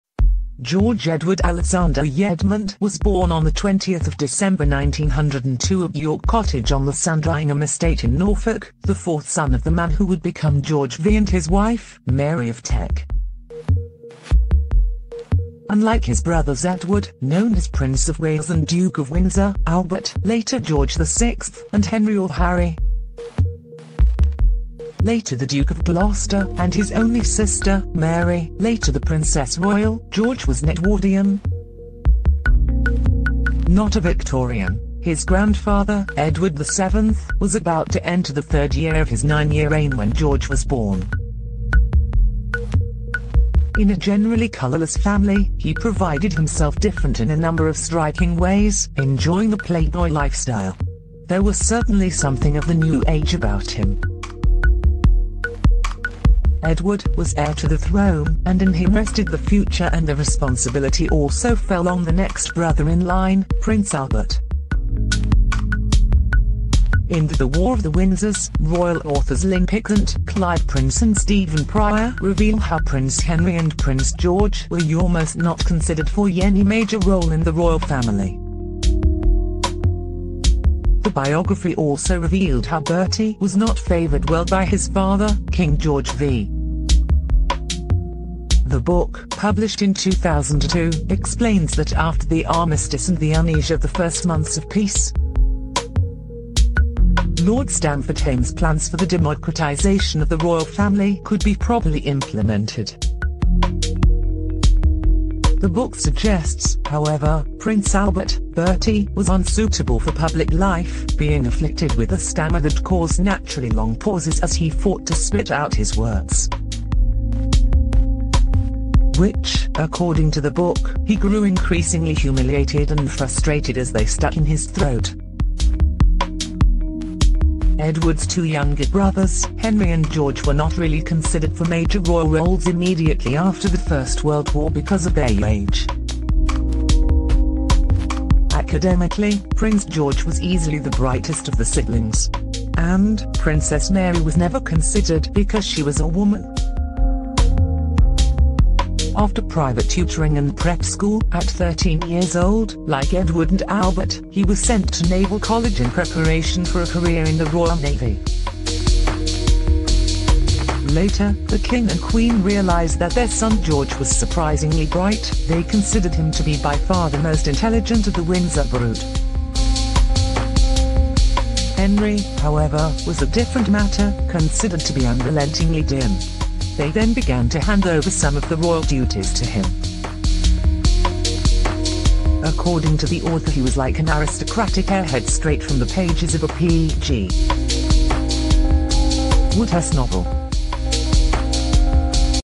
Royal REJECTION: How Prince Harry and Prince George were NOT considered for royal roles
PRINCE George, Duke of Kent, and Prince Henry, Duke of Gloucester, were the least favoured of the Windsor brood and were not considered for any major roles within the monarchy, a biography has claimed.